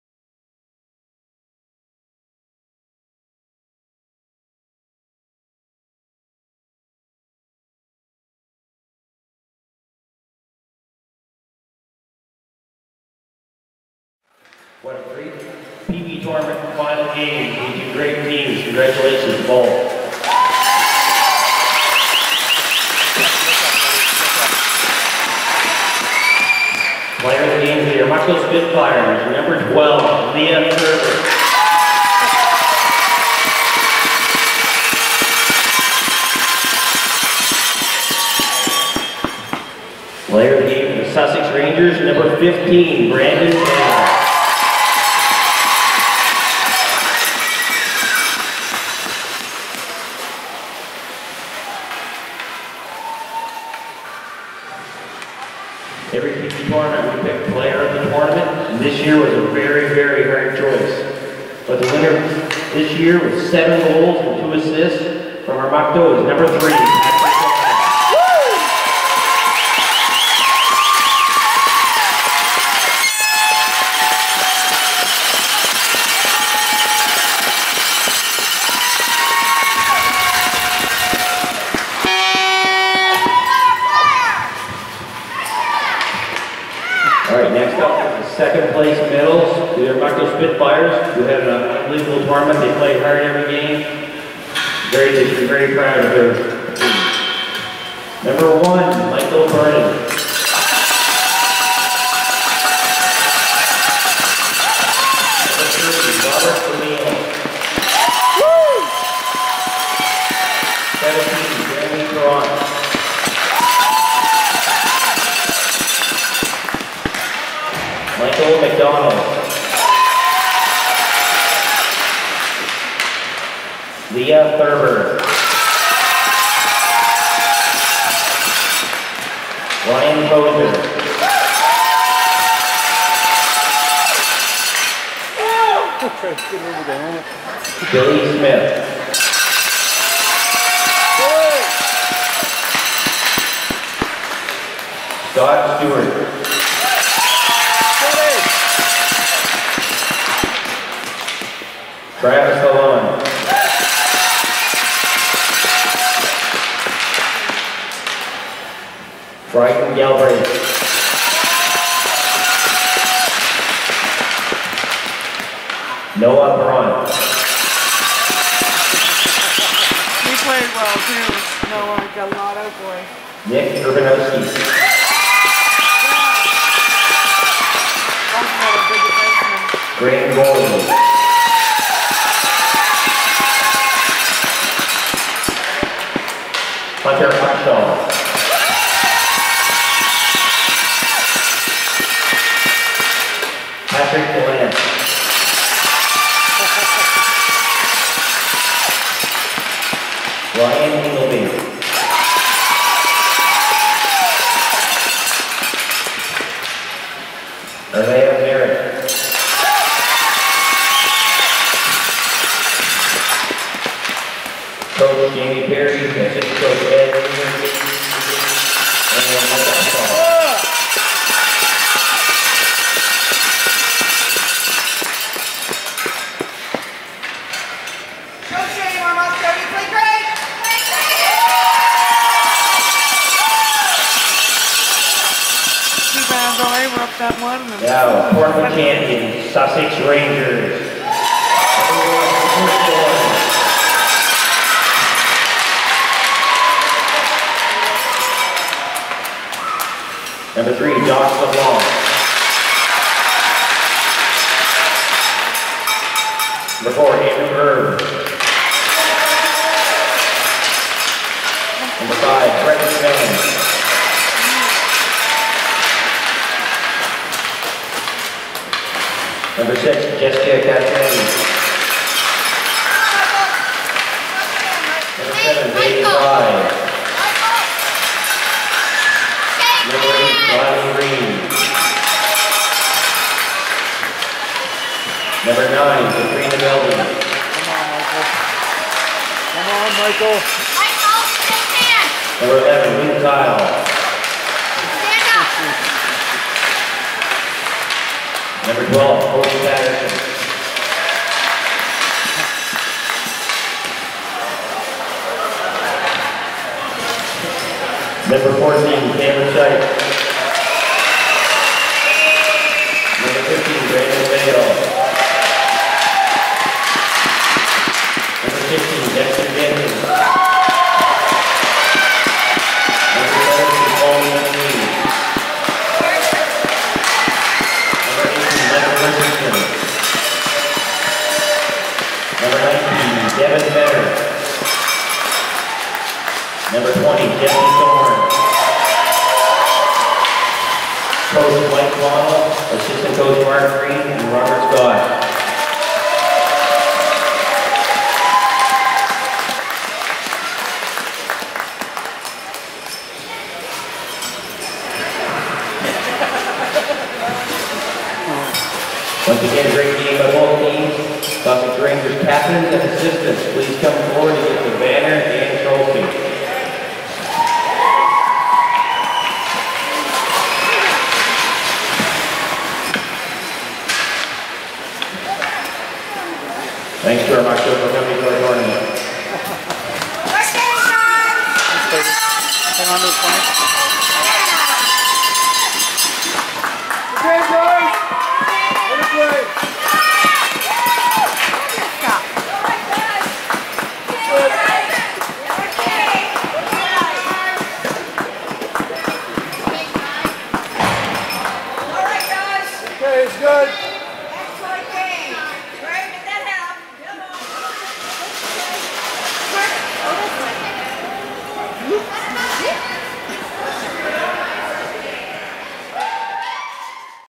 Trico Final - Part 5 of 5 - Postgame.wmv
Tri-County Tournament - Championship Final - Part 5 of 5 - Postgame presentations.